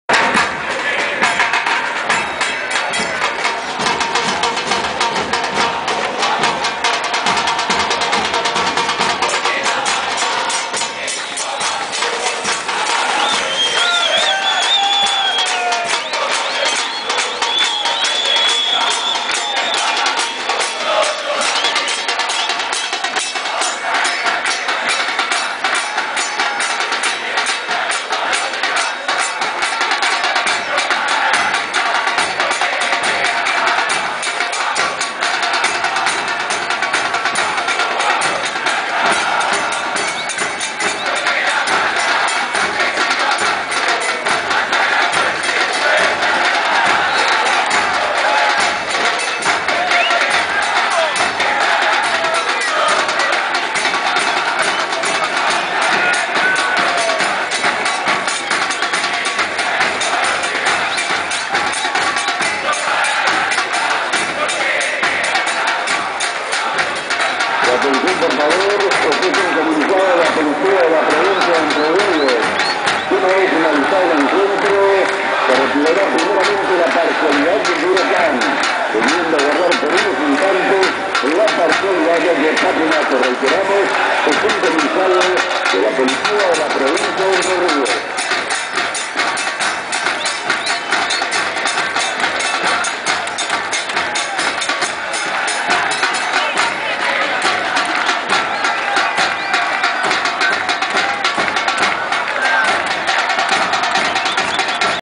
Patronato vs Huracán - Video IV - Huracán TV -
La hinchada del Club Atlético Huracán - Patronato 3 - Huracán 0.